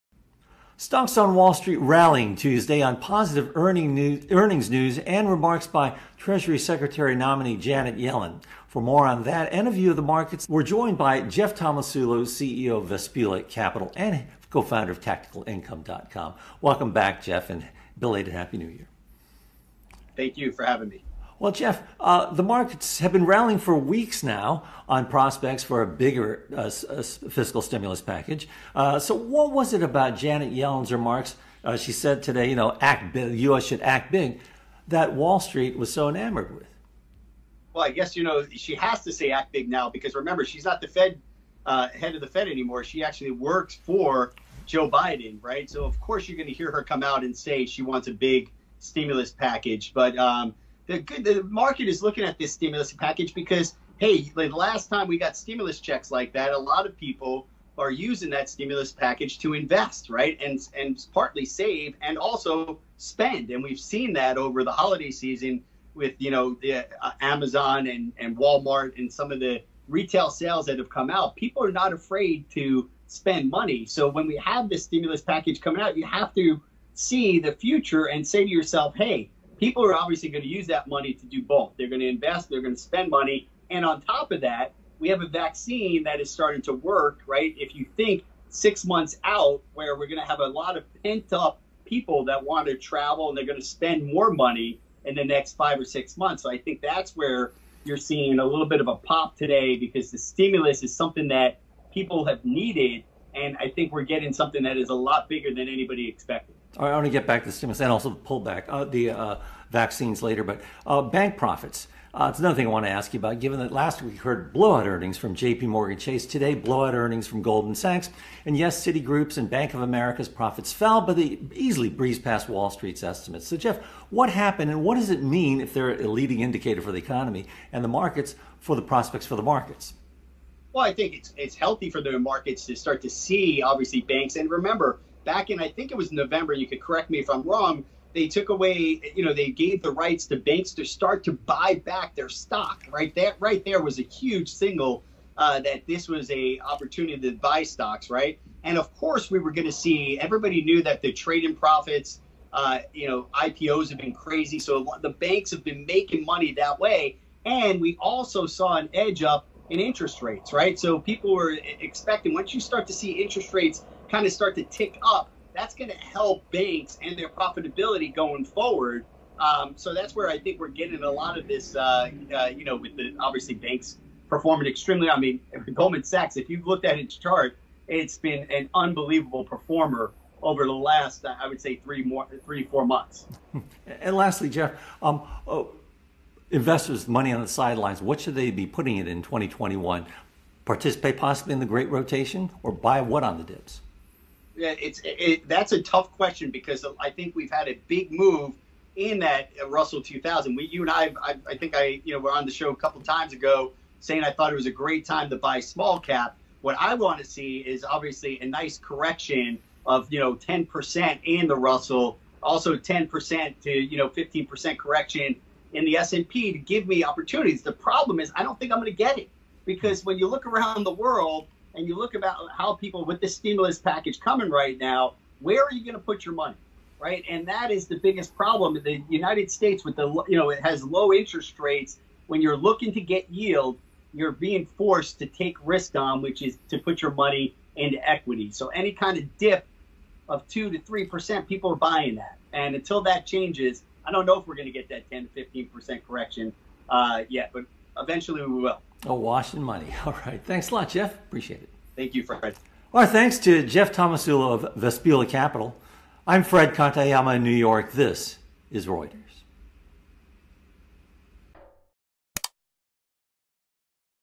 Big pullback won't come soon: trader - Personal Business Ideas
Personal Business Ideas

Big pullback won't come soon: trader

Vespula Capital's Jeff Tomasulo tells Reuters' Fred Katayama why he doesn't see a correction around the corner despite the equity markets' big run-up in the last quarter